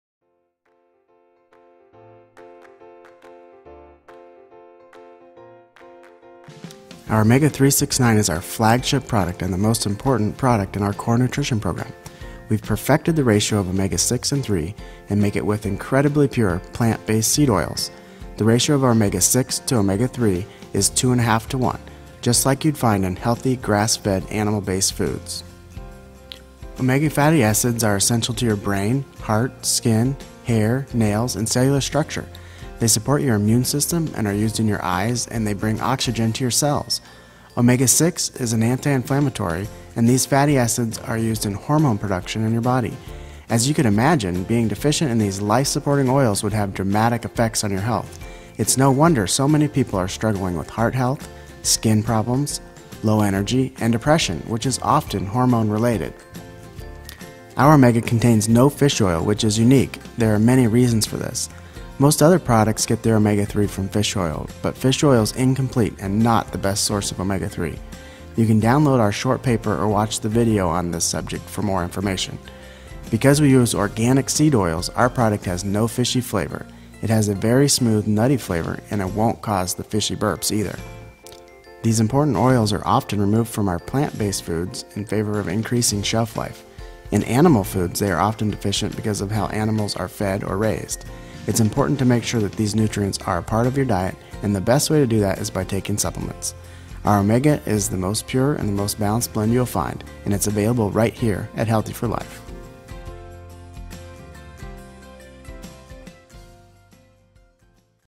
Omega 3-6-9
Omega 3-6-9 is scientifically formulated to conveniently replenish nutrients you may not be getting from your diet. Our exclusive formula uses a unique combination of organic oils to ensure lasting freshness. There are no additives or preservatives in our Omega Supplement. Omega 3-6-9 contains NO fish oil, for maximum purity.